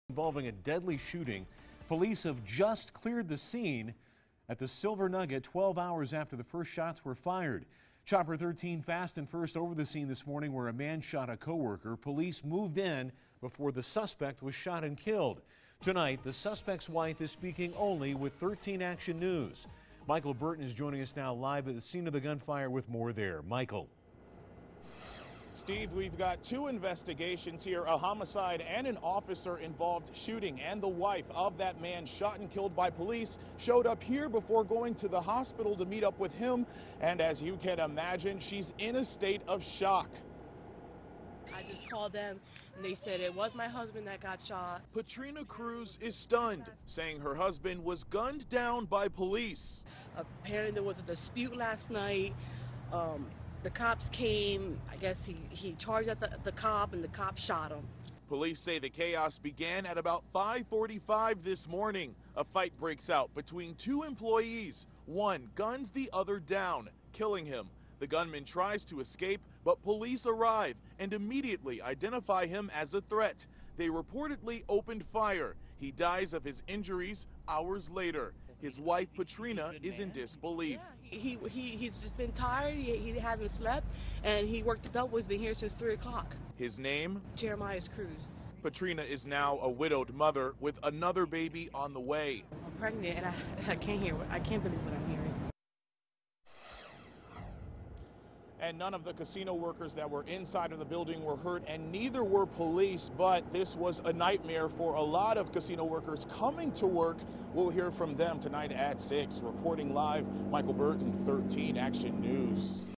UPDATE: 2 people dead after incident at North Las Vegas casino
One victim is dead following a homicide, and one suspect is dead after an officer-involved shooting.

On May 19 around 5:43 a.m. officers responded to the Silver Nugget casino in the 2100 block of Las Vegas Boulevard North in response to a violent battery in progress.